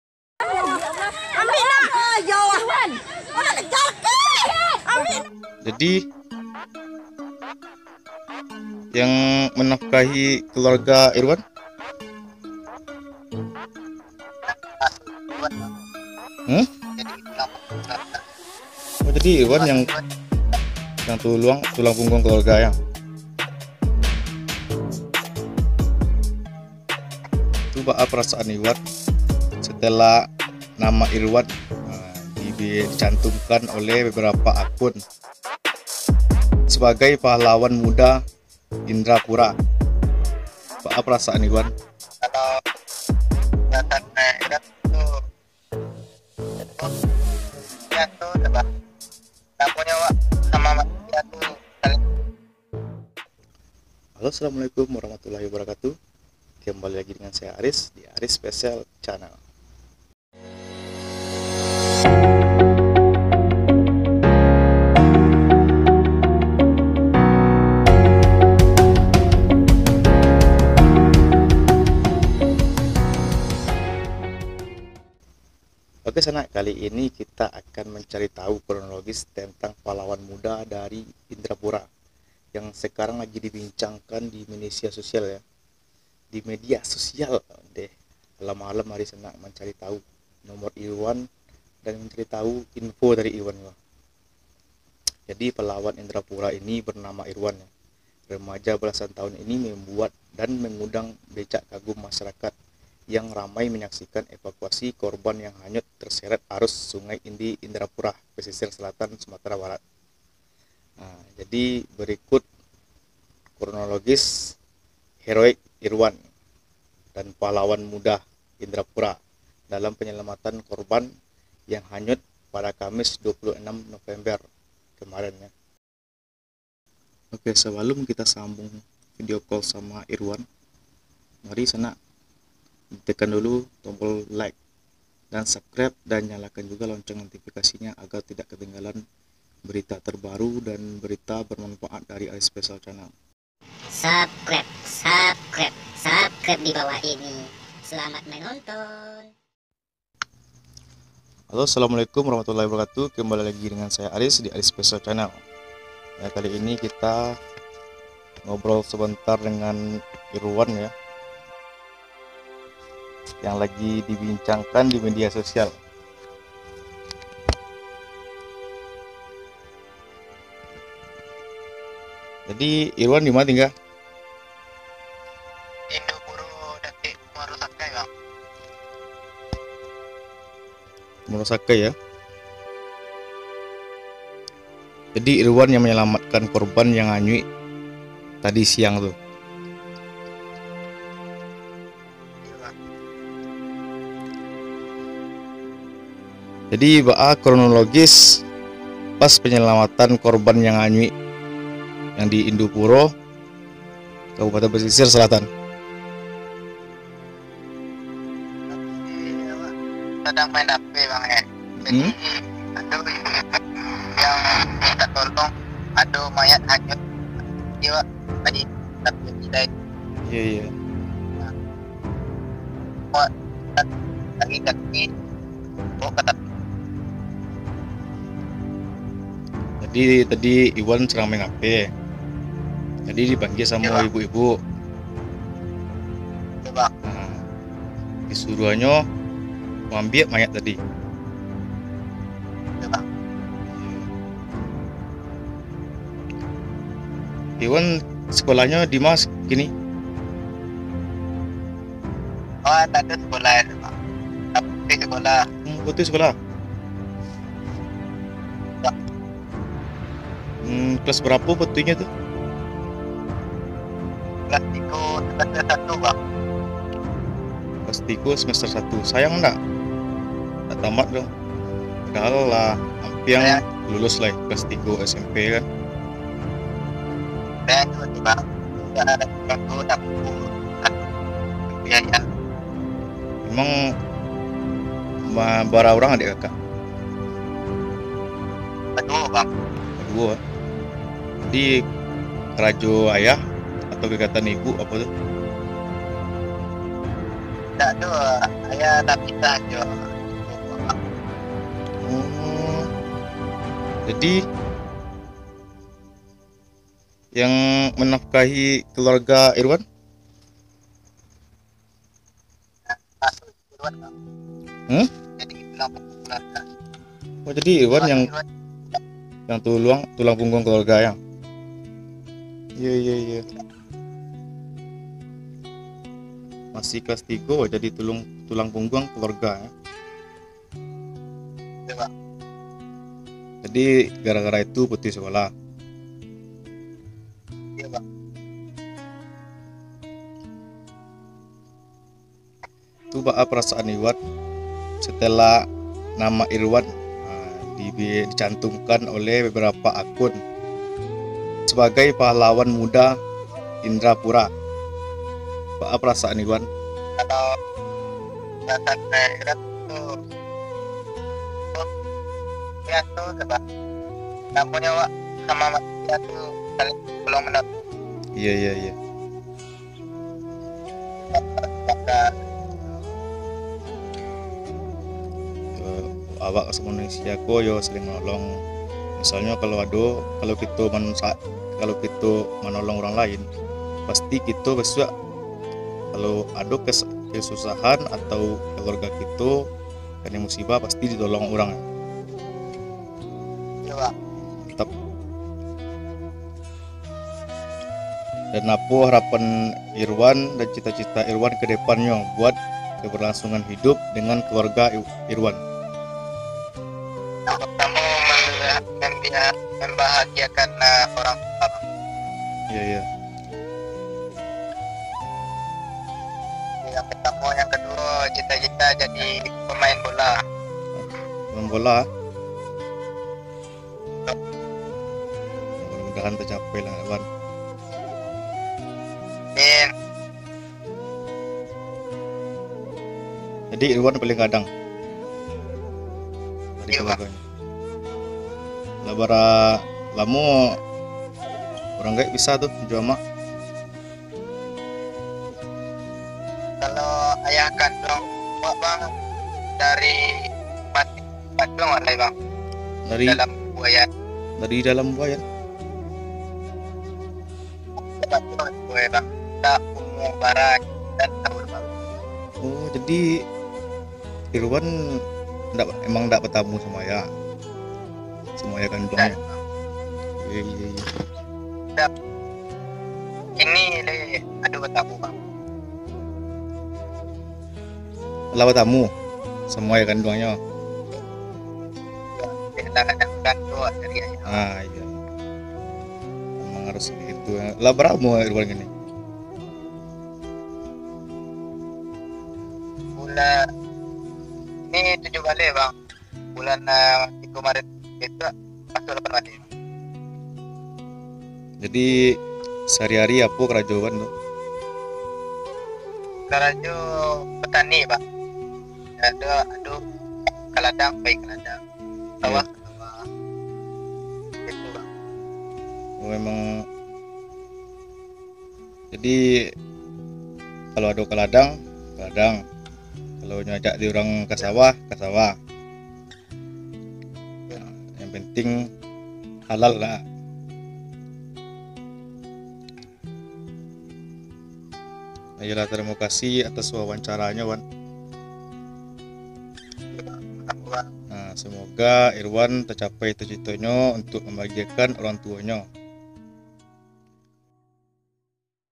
irwan inderapura penyelamatan korban hanyut, di juluki sebagai PAHLAWAN MUDA INDERAPURA, pessel
Aksi heroik penyelamatan korban hanyut, di juluki sebagai PAHLAWAN MUDA INDERAPURA, pesisir selatan.
ARIS PESSEL

-coment
GRATIS!! TIDAK DI PUNGUT BIAYA

HAPPY LISTENING AND HAPPY ENJOOY..
Salam rang pasisia